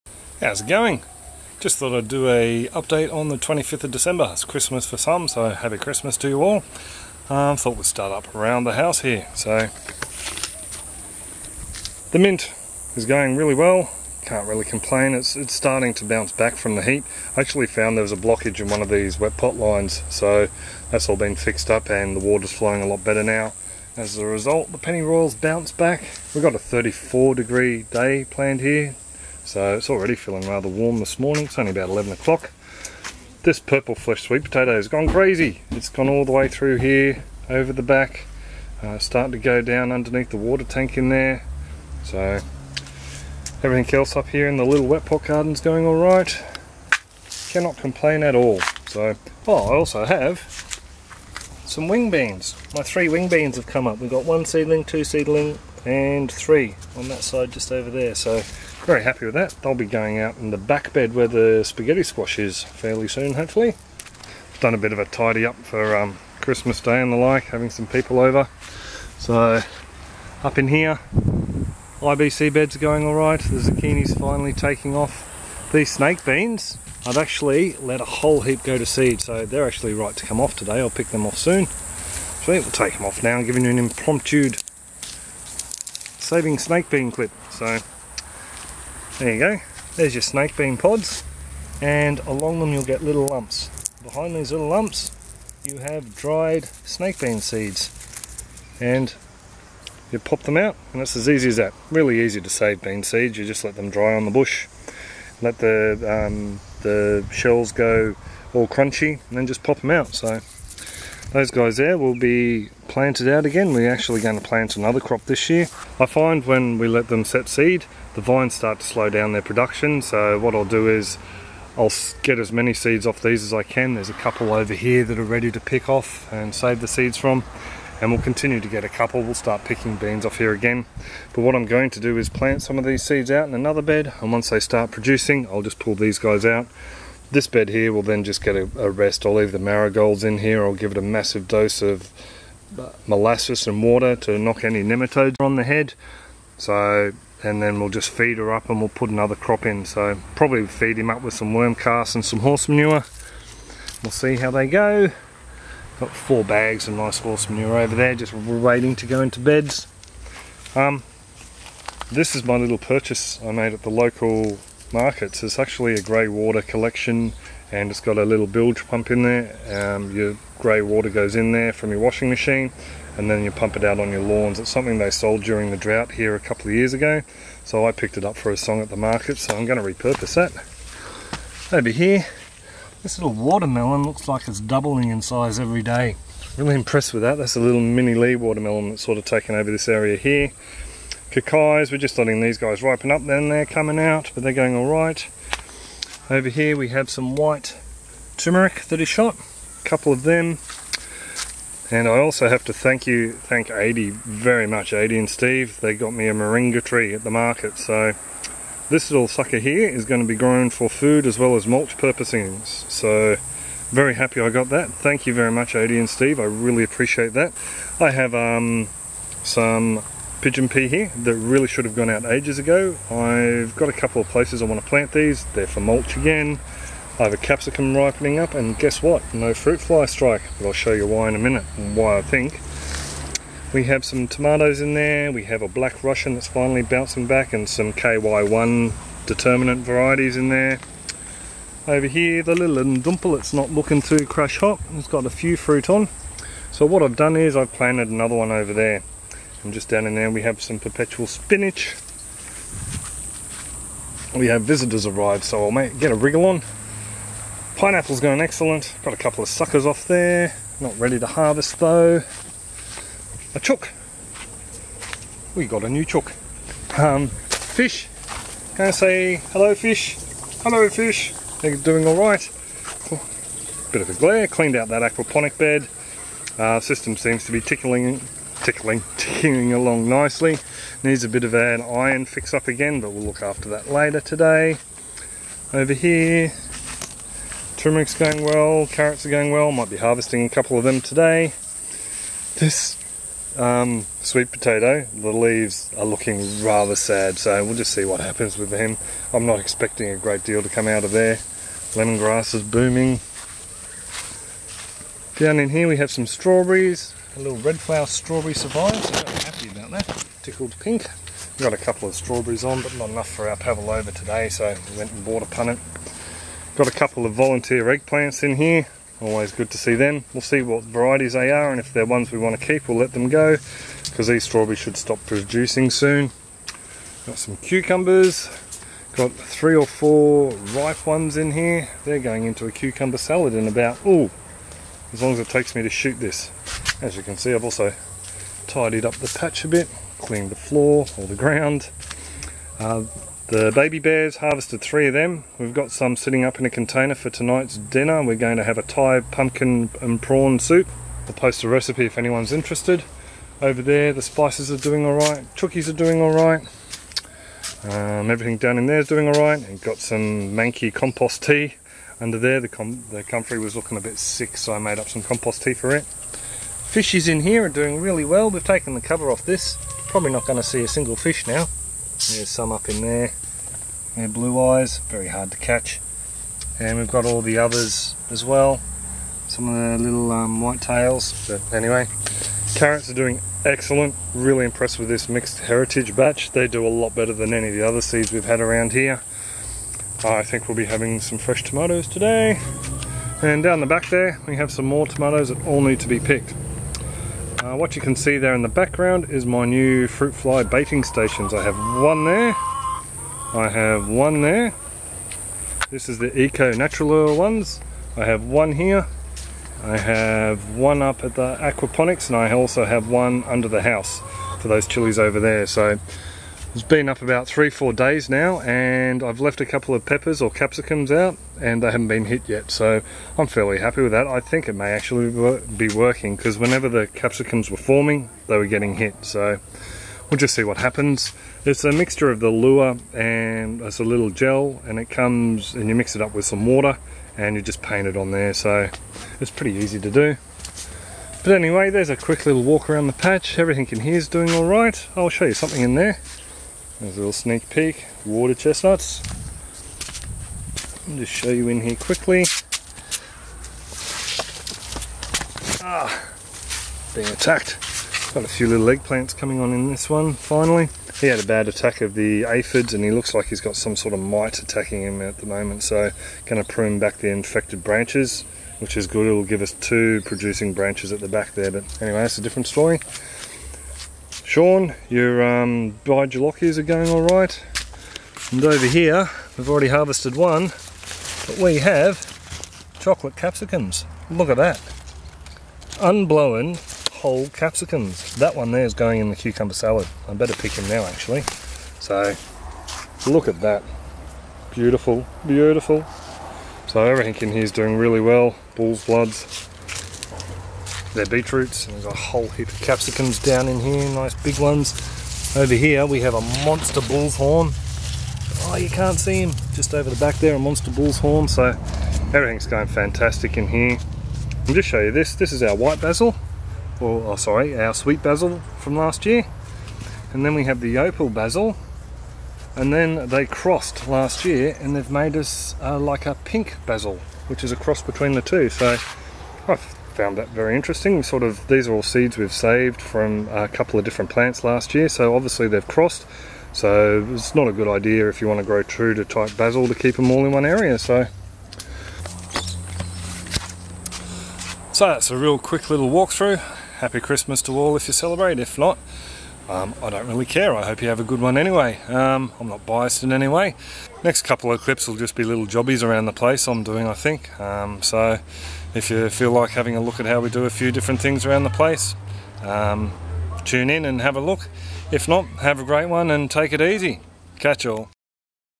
Veggie garden walk through & saving a few snake bean seeds 25th December 2012..
Was hoping to get this up yesterday but time got away from me..
Hope everyone had a great one..

For more regular updates about the patch come visit us